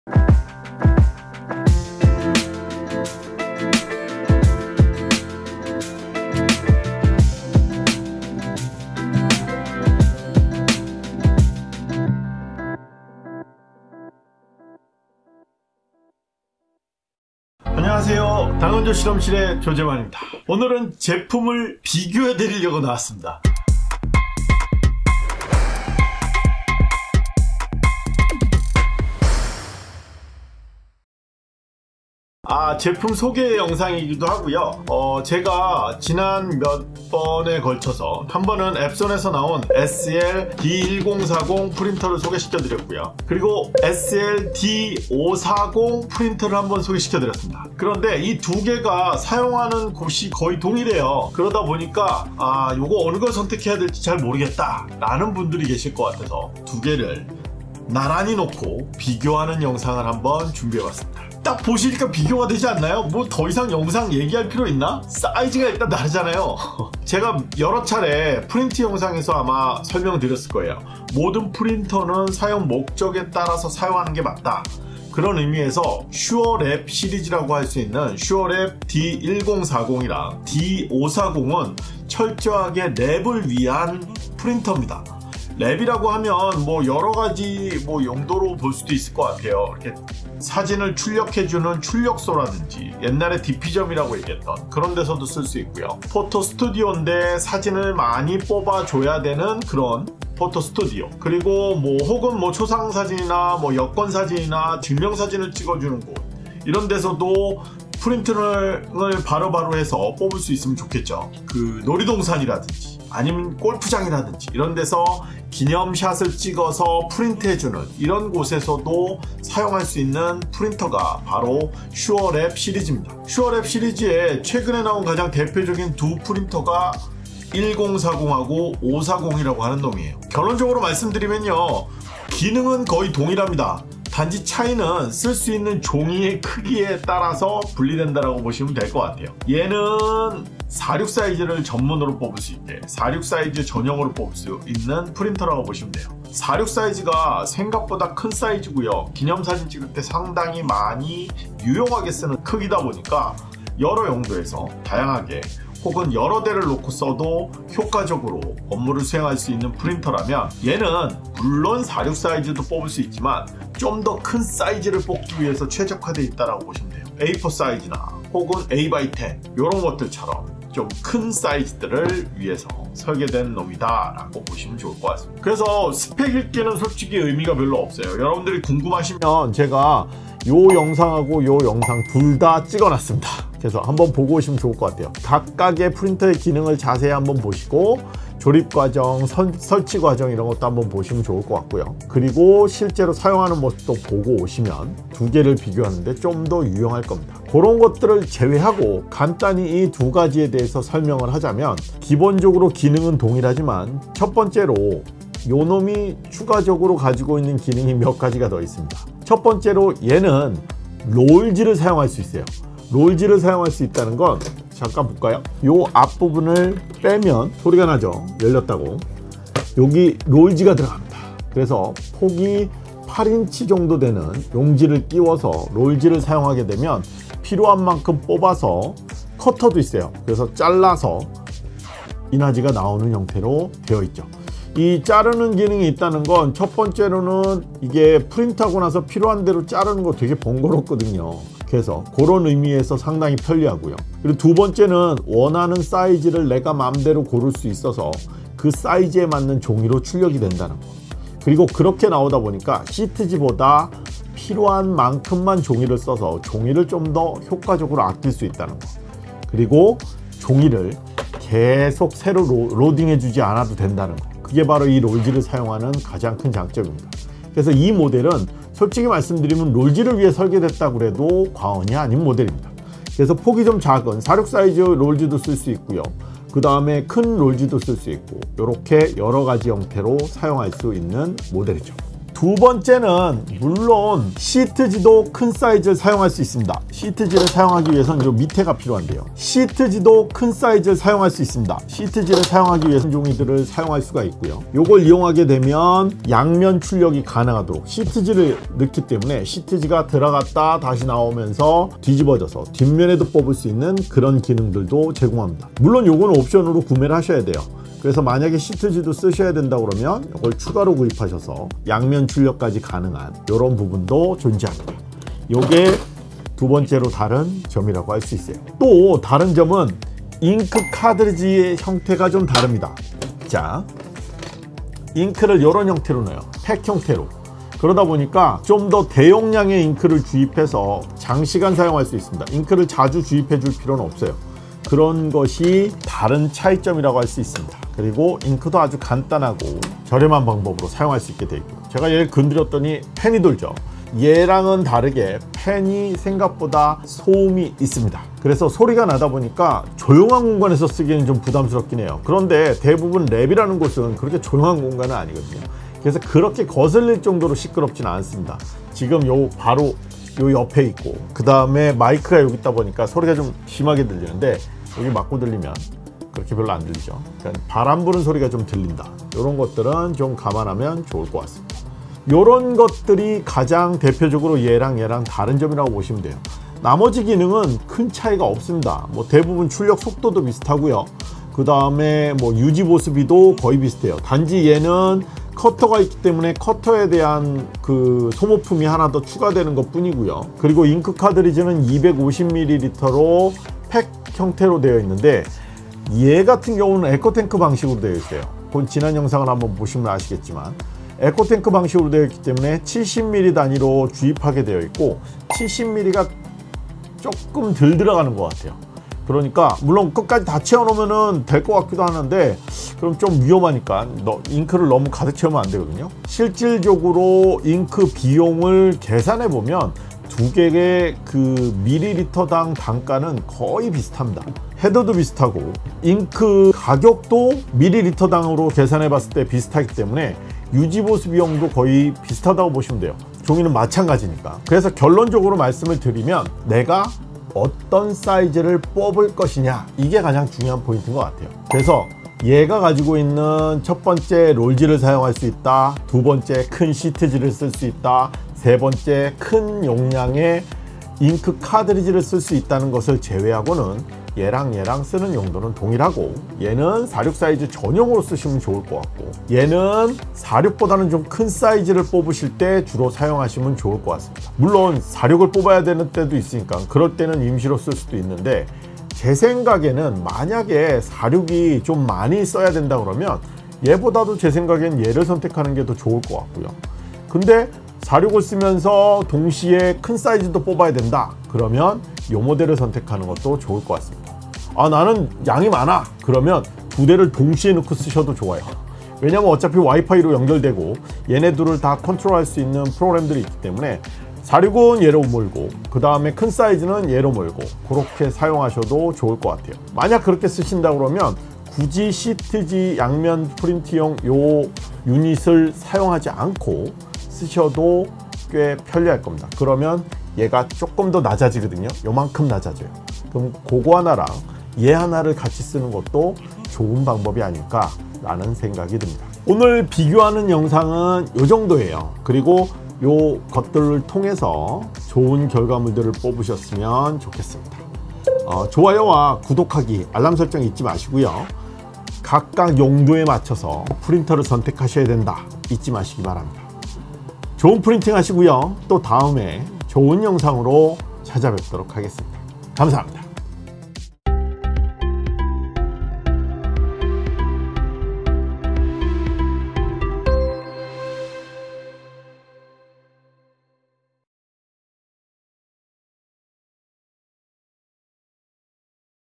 출력소, 프로사진가, 인생네컷을 위한 프린터 선택 [가성비갑쓰] 엡손 SureLab D1040 vs D540 비교 영상
오늘은 두가지 프린터의 비교 영상입니다.
엡손의 프린트 랩을 위한 대용량 대량 출력을 위한 프린터 SL-D1040 과 D540의 비교를 해 보겠습니다.

0:00        intro
0:32        영상에 대한 소개
1:01        비교하자 슈어랩 SureLab 시리즈의 사용처
3:30        SL-D1040 의 특징
6:30        어느걸 선택할까요
8:40        마무리

입니다.